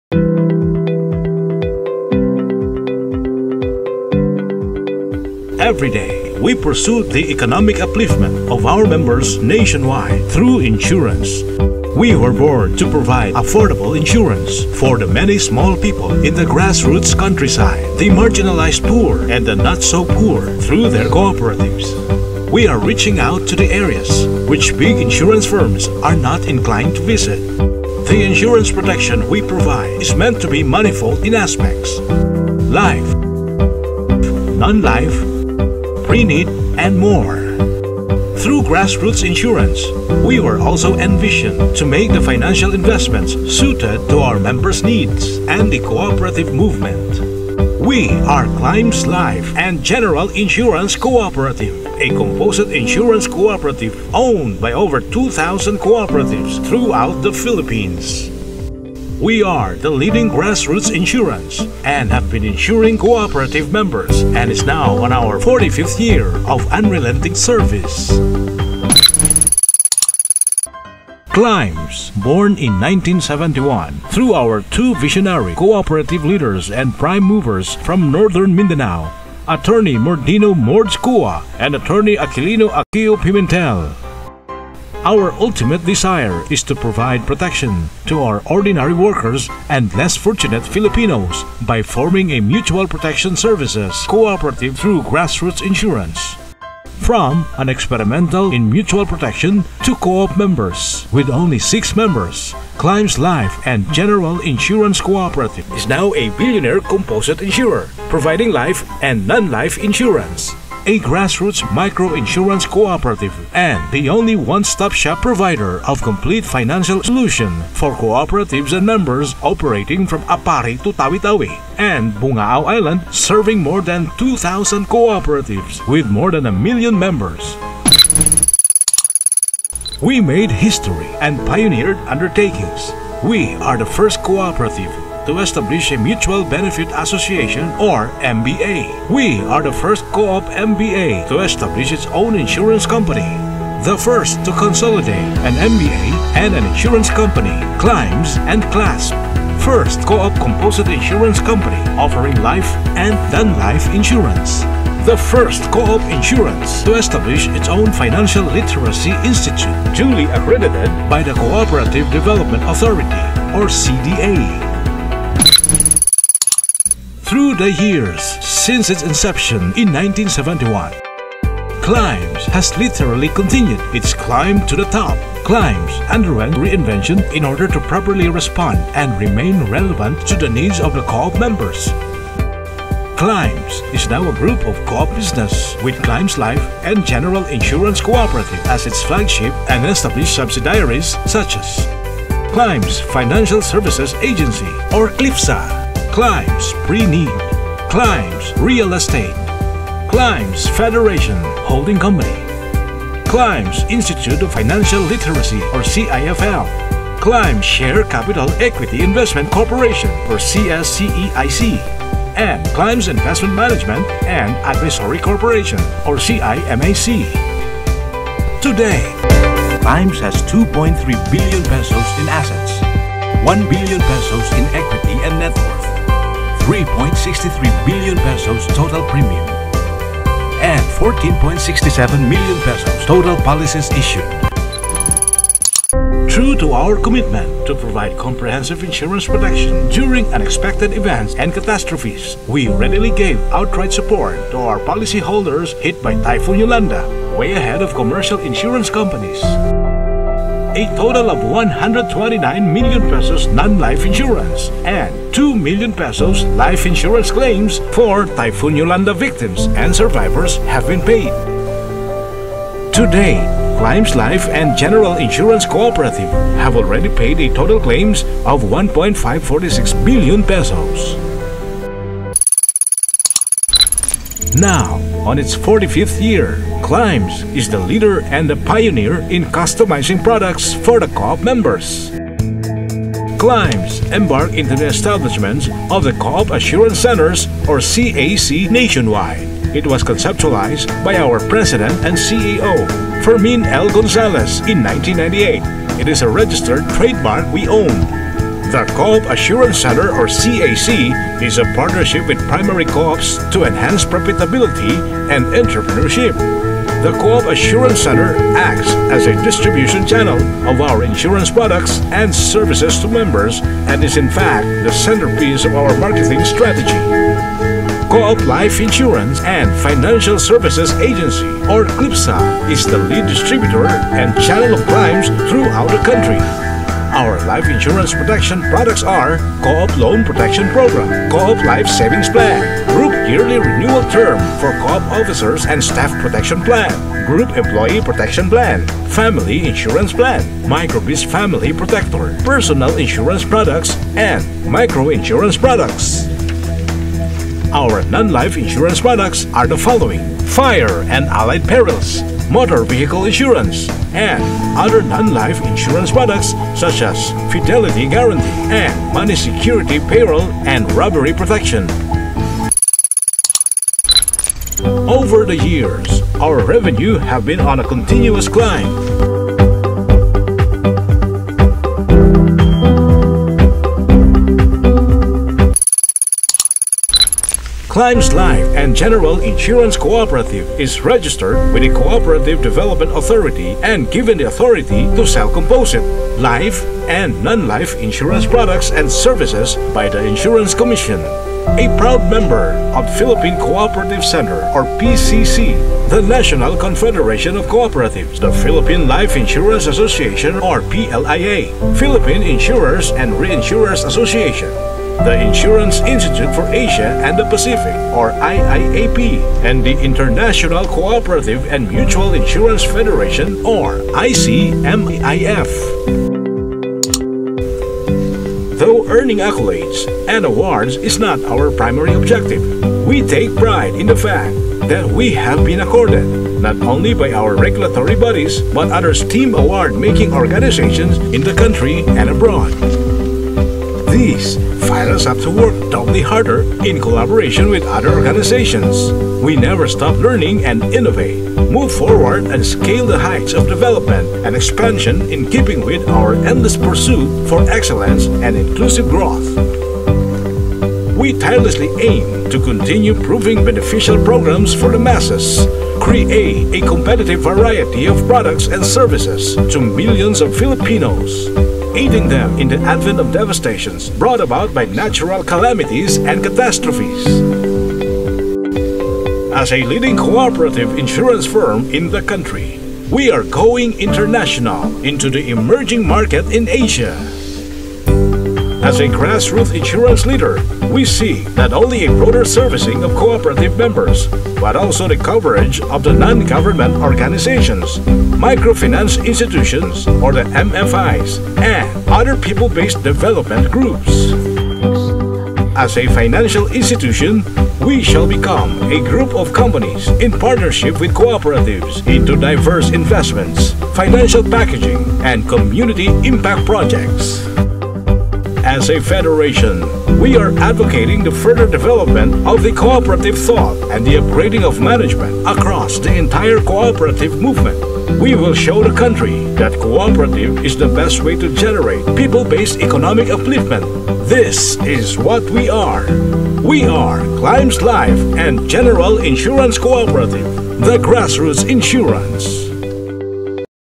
CORPORATE VIDEO OF CLIMBS INSURANCE
This is a corporate video of CLIMBS Life and General Insurance Cooperative produce by Cooperative Broadcasting Federation of Mindanao